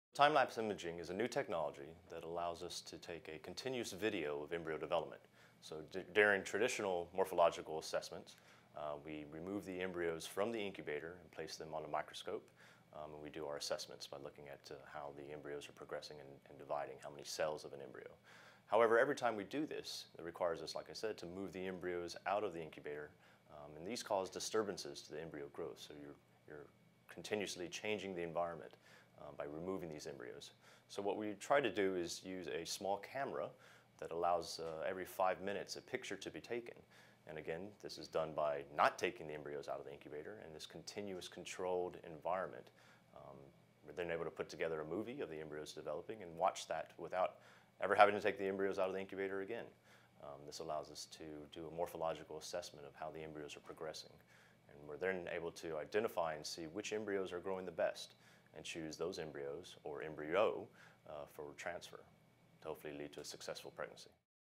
Embryo Grading with Timelapse Imagery
Scientific Director, Matthew "Tex" VerMilyea discusses the benefits of embryo assessement with timelapse imagery.